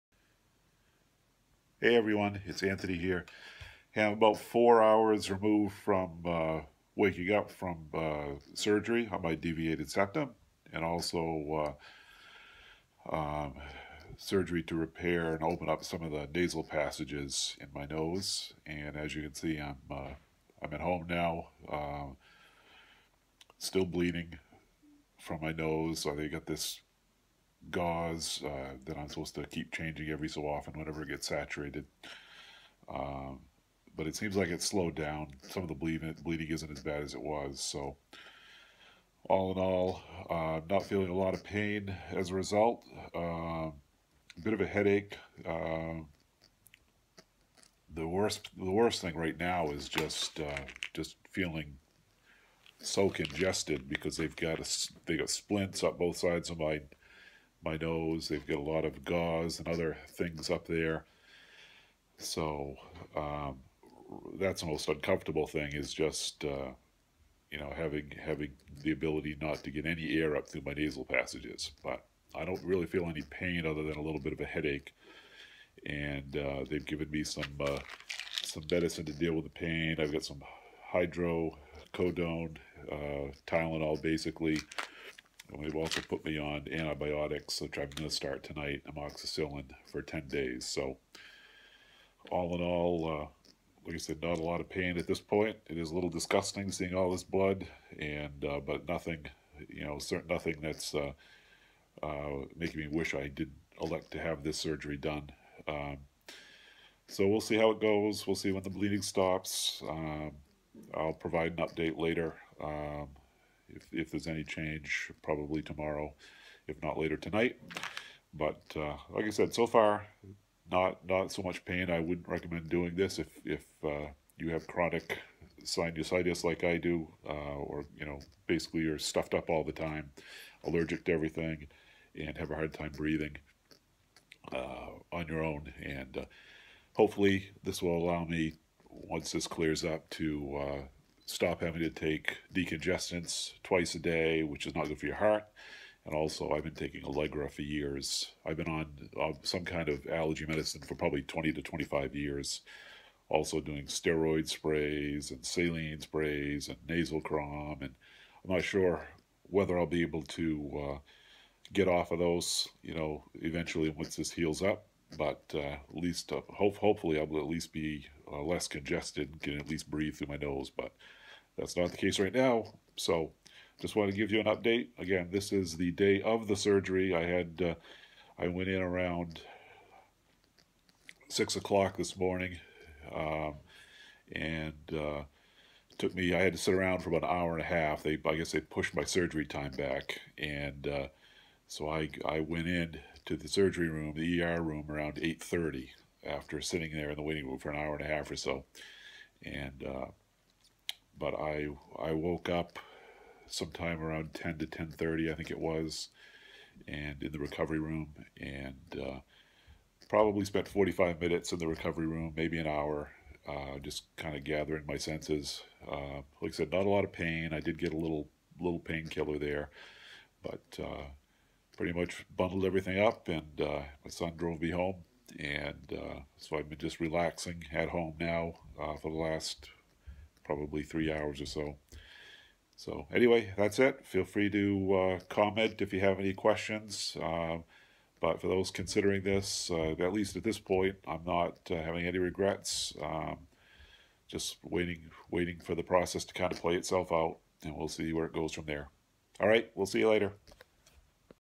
Deviated Septum (Septoplasty) & Turbinate Reduction Surgery - 4 Hours After Surgery
Four hours after Deviated Septum (Septoplasty) and Turbinate Reduction Surgery.  No significant pain at this point, although I have a bit of a headache.  Bleeding is still taking place, although it has slowed down some since I was in recovery.  Although I'm very used to being congested, my nasal passages are now much more plugged up due to the material placed there to soak up the blood and encourage clotting to take place.